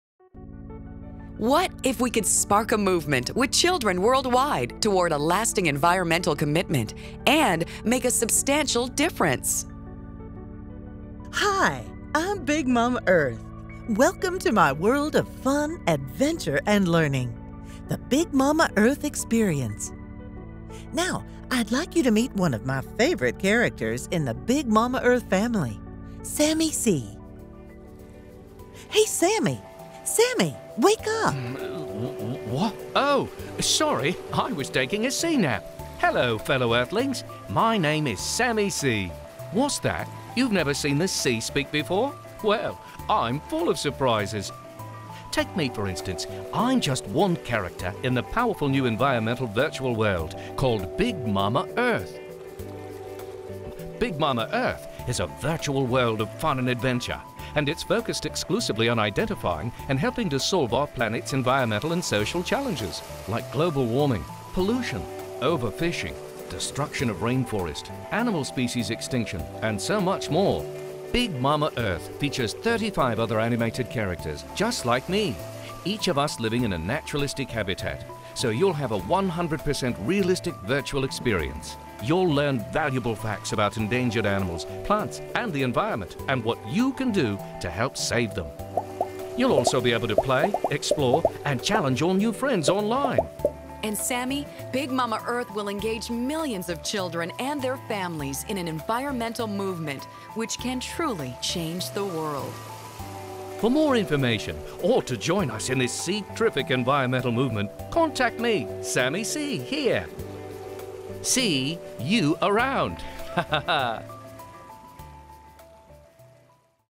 Big MaMa Earth - Join the Movement - Please Donate
Big MaMa Earth will be a new and exciting environmental virtual world for children that will focus on such critical issues as: climate change, health of our oceans, seas, lakes and rivers, pollution, rain forest destruction, animal species extinction and endangerment, and many more.   Big MaMa Earth and her 36 animated character friends may be the catalyst to further empower children and ourselves to take responsibility, to change our patterns, from governments to individuals.  Re-connect to Mother Earth and join the movement

We formed a new non-profit 501 (c)(3) childrens learning academy, the Big MaMa Earth Learning Academy. (BMMELA)

Be part of the change. We are seeking donations from the public, individuals, groups, corporations and other foundations & non-profits to expand our learning academy website and lessons and messages that will be brought forth from the top educators, scientists and renowned experts in their fields.

We also ask that you PLEASE DONATE to the BIG MAMA EARTH LEARNING ACADEMY.

YOUR FUTURE IS NOW!!